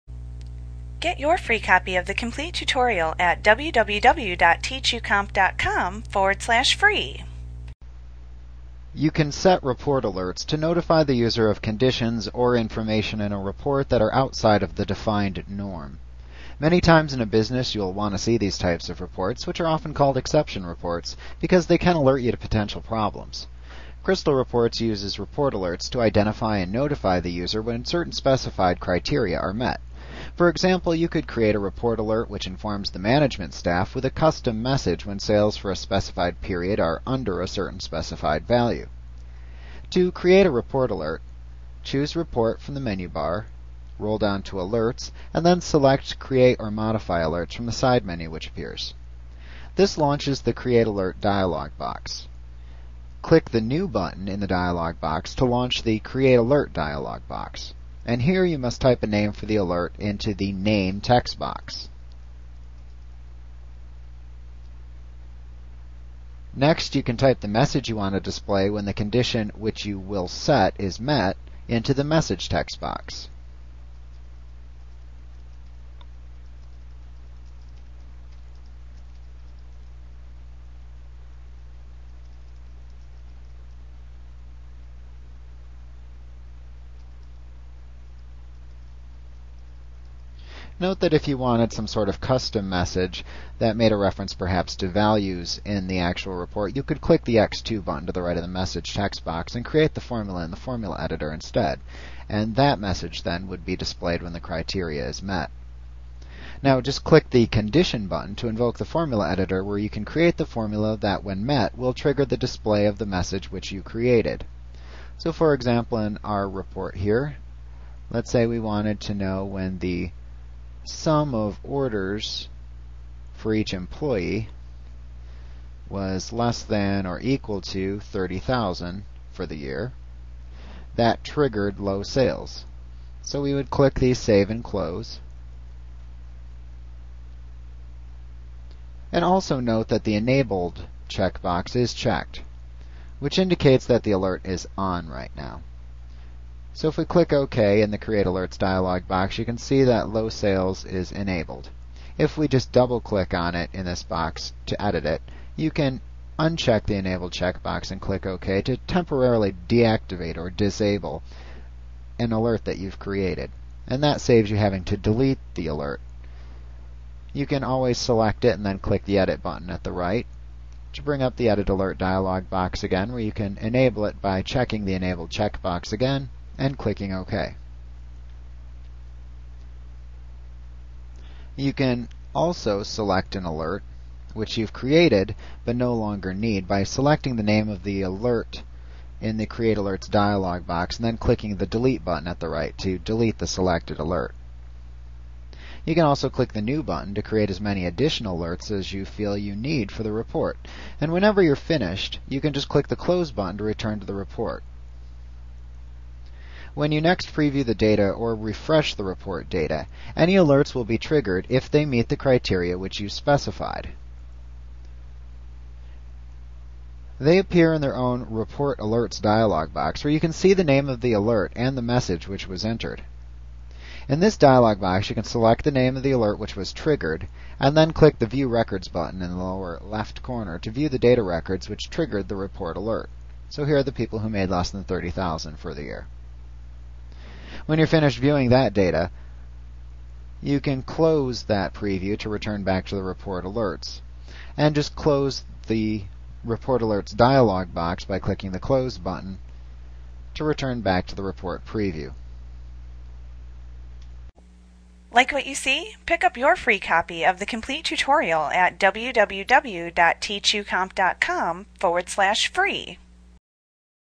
Crystal Reports Tutorial Report Alerts Business Objects Training 13.9
Learn about report alerts in Crystal Reports by Business Objects A clip from Mastering Crystal Reports Made Easy v. 11. - the most comprehensive Crystal Reports tutorial available. Visit us today!